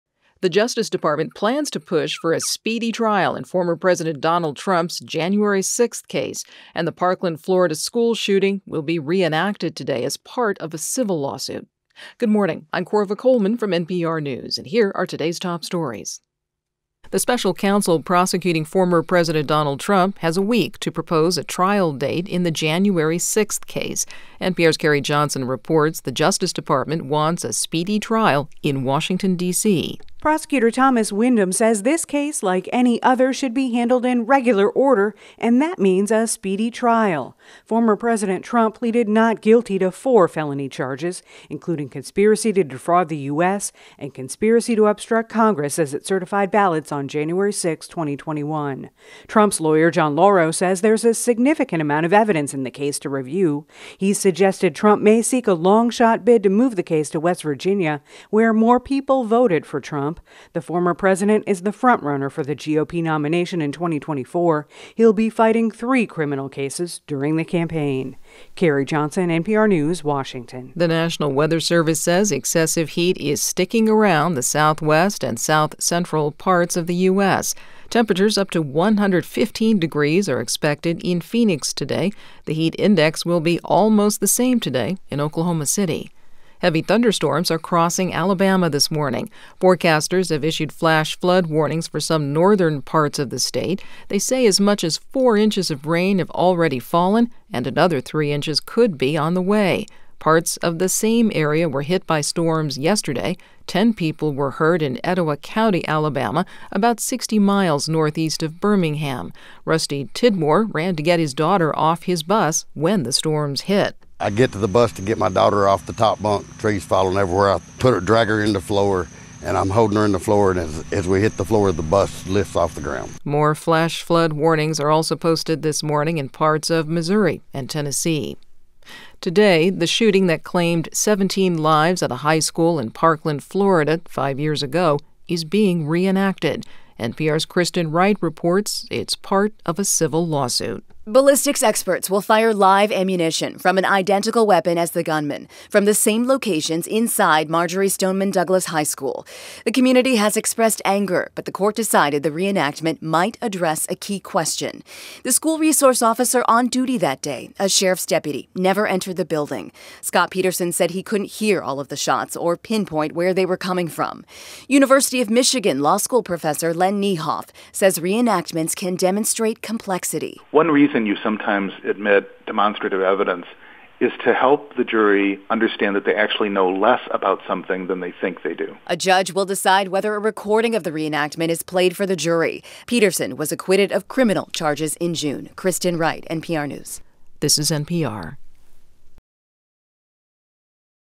Trump Pleads Not Guilty To Charges Linked To Capitol Attack | NPR News Now
NPR’s “News Now” is a roundup of the top stories of the day in under five minutes. Click “Show More” for a list of today’s stories:

0:00 Today’s Top Stories
0:16 DOJ Pushing For Speedy Trial In Trump's Latest Case
1:10 Intense Heat Continues To Scorch Parts Of The U.S.
1:26 Waves Of Rain Inundating Northern Alabama
2:09 Parkland School Shooting Reenacted Today

Cover Photo: Dana Verkouteren via AP